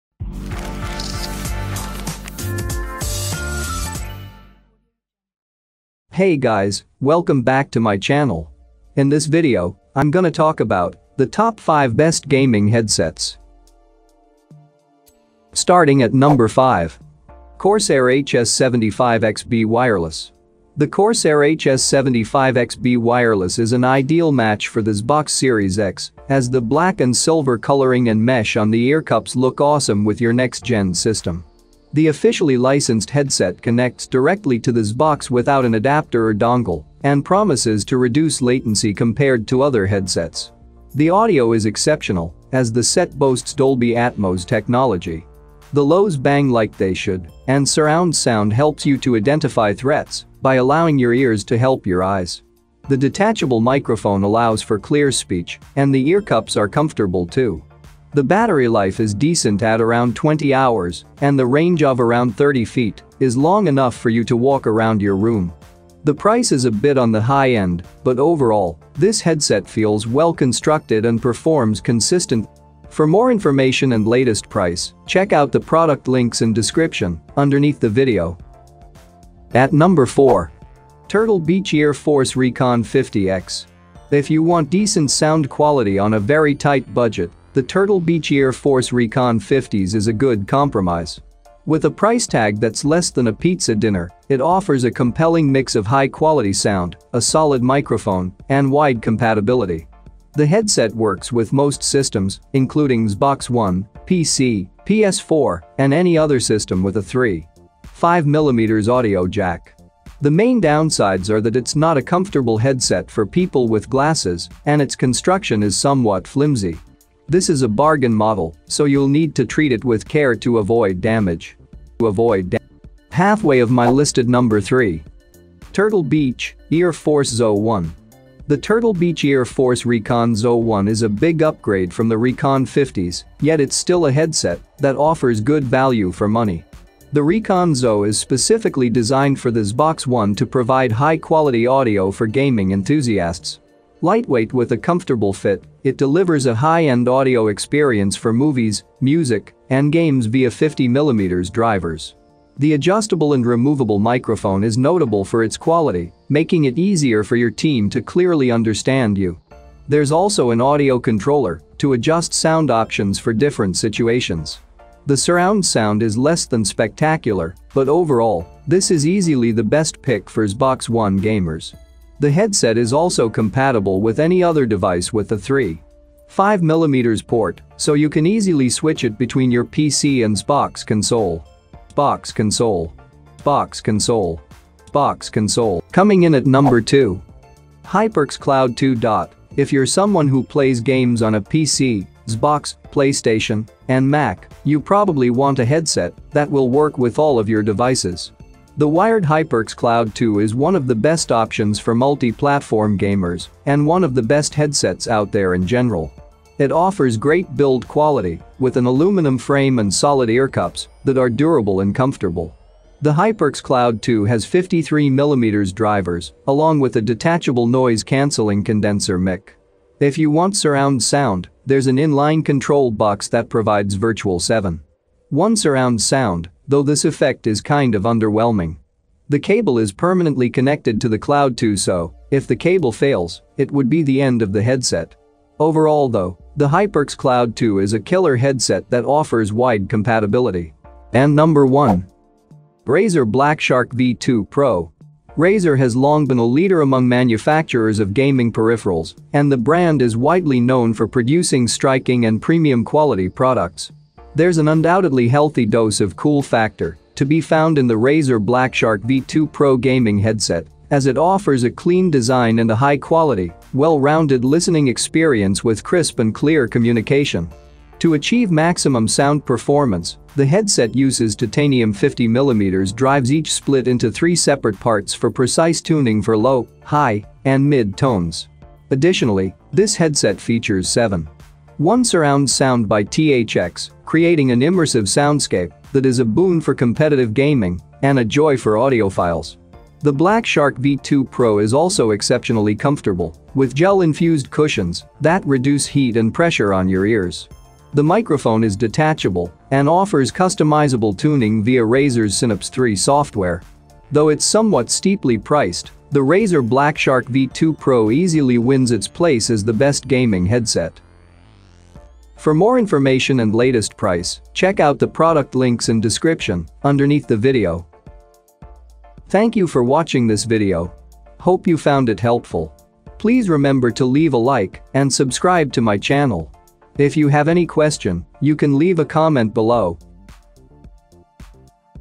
Top 5 Best Gaming Headsets in 2023
Here is the Updated Prices of the Best Gaming Headsets in 2023!

Things that we mentioned in this video:
1. Razer BlackShark V2 Pro - B08FQG96RP
2. HyperX Cloud II - B00SAYCXWG
3. Turtle Beach   Ear Force XO One - B00KSVXSZU
4. Turtle Beach Ear Force Recon 50x - B00YXO5U40
5. Corsair HS75 XB Wireless - B08JD18Q4Q